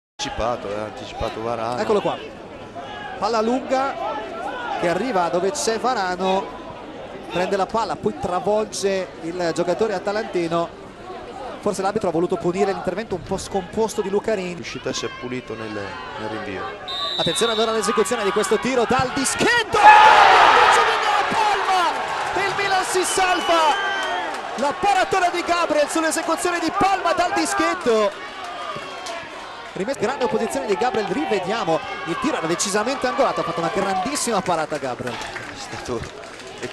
Gabriel Saving a penalty vs Atalanta
Milan - Atalanta - 2-1
[GANZ]
[PETAGNA]